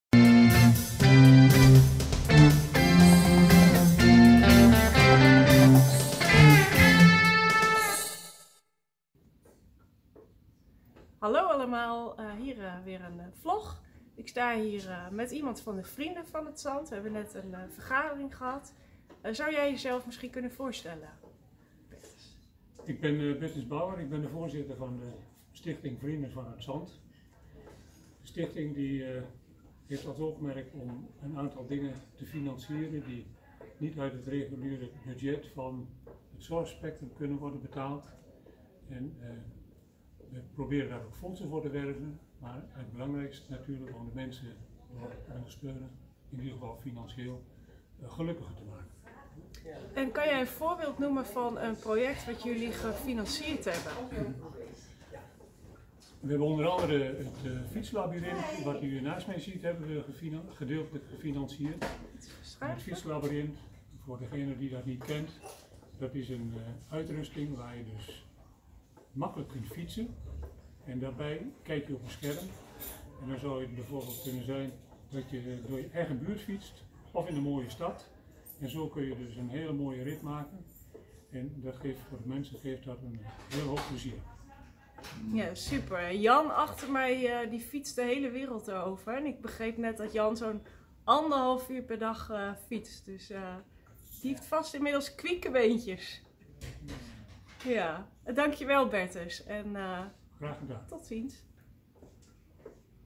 Stichting Vrienden van Het Zand
Van een rondje fietsen door het dorp tot boontjes kweken in de moestuin. Geluk zit 'em in de kleine dingen. De Stichting Vrienden van Het Zand dragen daar graag aan bij. Benieuwd naar wat ze doen? Bekijk deze vlog of ga naar de